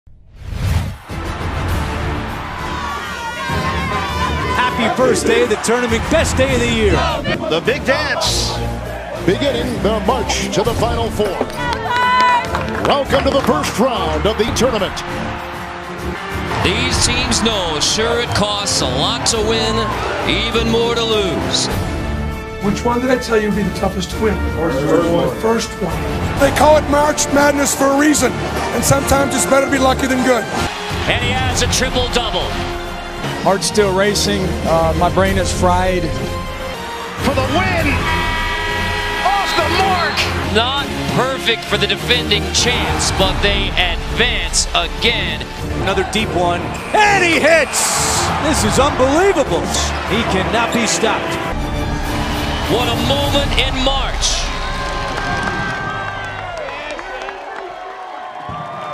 March Madness Kicks off at Service Express
March Madness is always a fun time at Service Express. Time to see which department will bring home the Title.